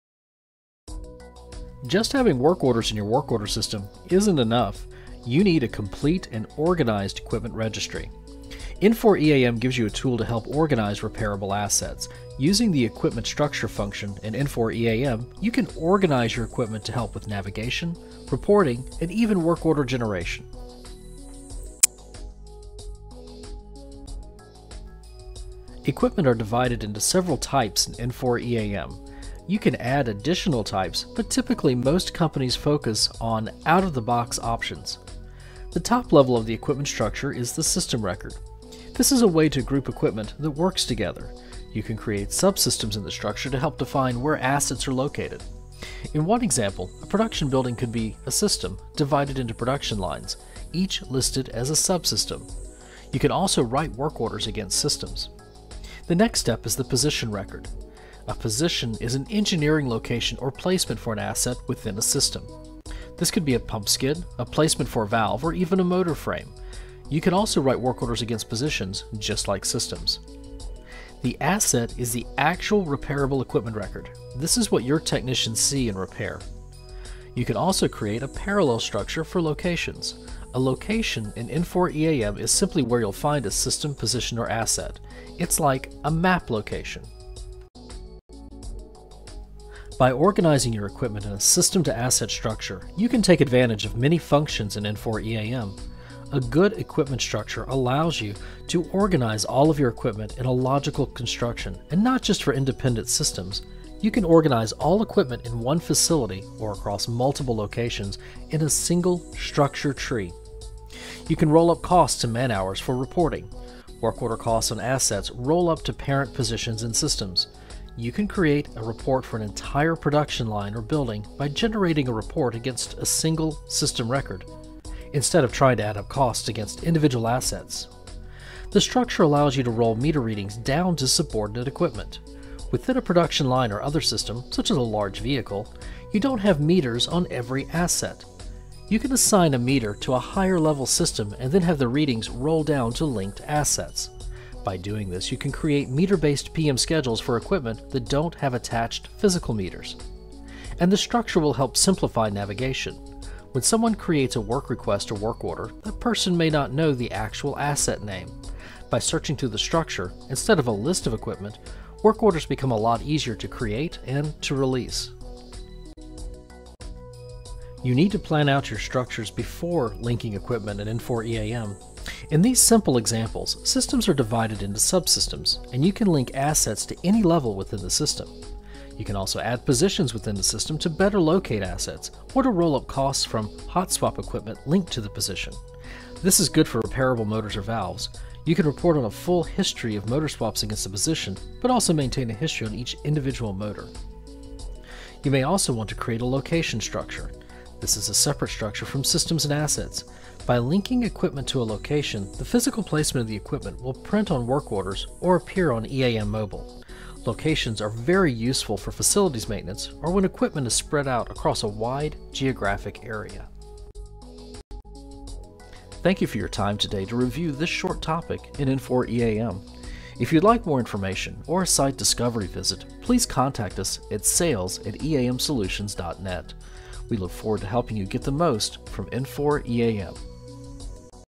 Connecting Equipment in Infor EAM
Having a work order system isn't enough - you need a good equipment list - or asset registry - to backup your work orders and PM schedules. EAM Solutions can help your group organize a great Equipment Structure in Infor EAM.